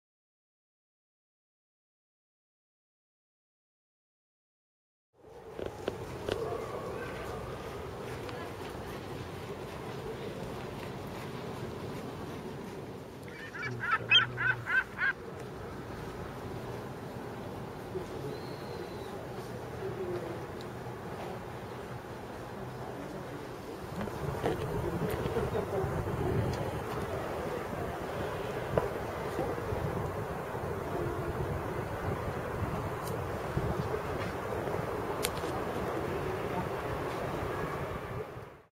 View from Okazaki Castle
Two angles from the top of Okazaki Castle